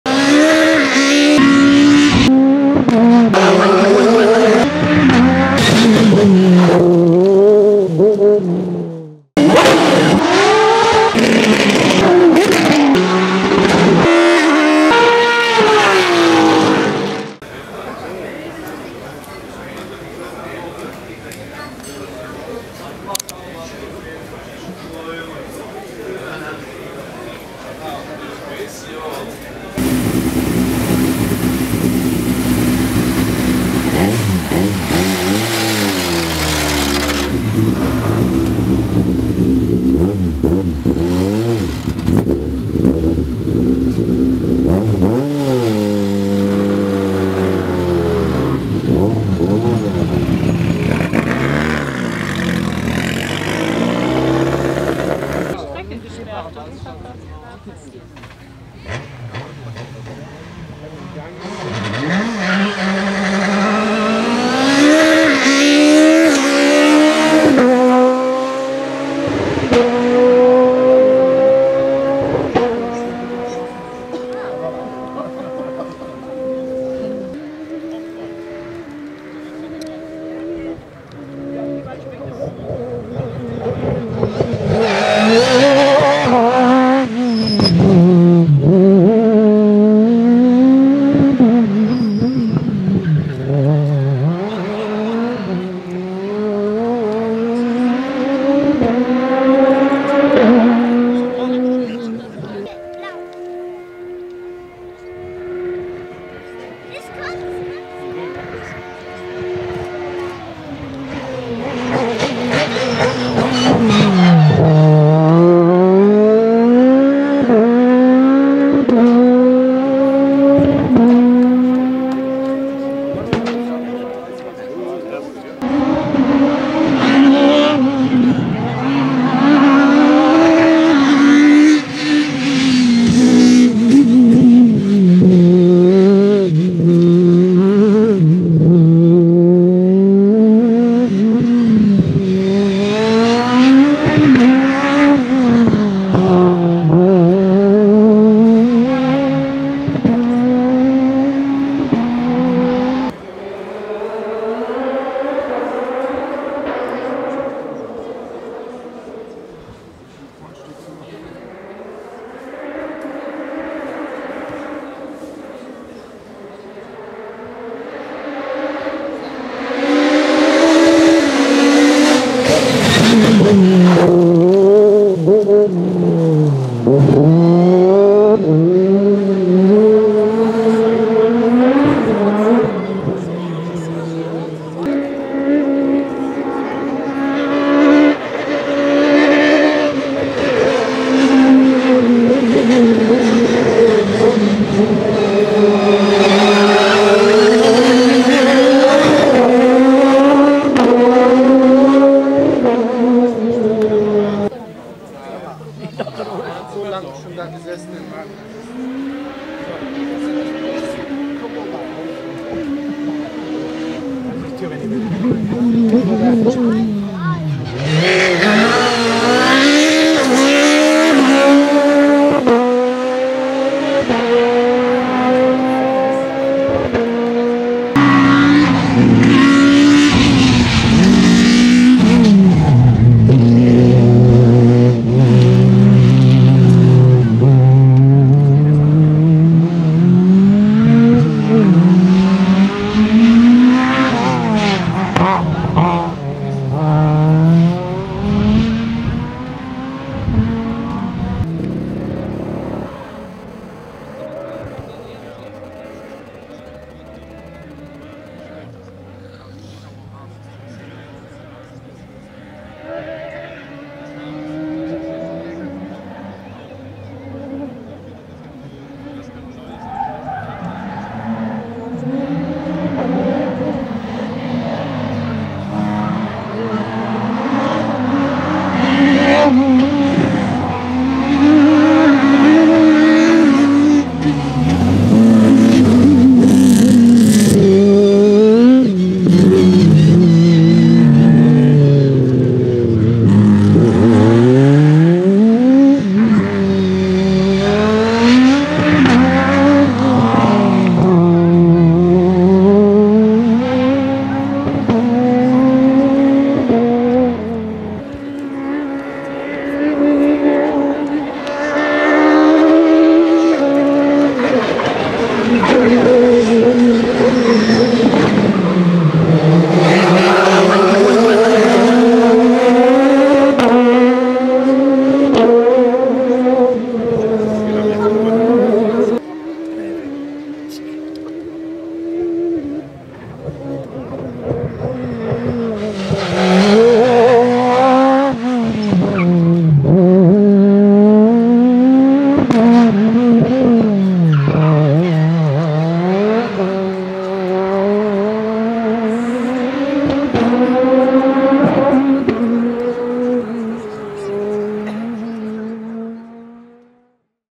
PEUGEOT 306 MAXI with Neuville | High revving, acceleration & action | Eifel Rallye Festival 2022
Engine: PSA XU10J4RS / inline-4 / 1998 cc / 121.8 cu in
Power: 280-300 hp / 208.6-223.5 kw max 11.000 rpm
Torque: 265 NM / 195.4 ft lb @ 8900 rpm
Weight: 960 kg /2116.4 lbs
Drive: FWD / 6-speed sequential

Event: Eifel Rallye Festival
Location: Daun
Date: 21-23 july 2022

Car nr.2 Thierry Neuville (BEL) / Antoine Cornet (BEL)
Car nr.73 Erich Müller (AUT) / Stefan Zabering  (AUT)

Filmed with
Canon Legria HF G40 + DM-100 microphone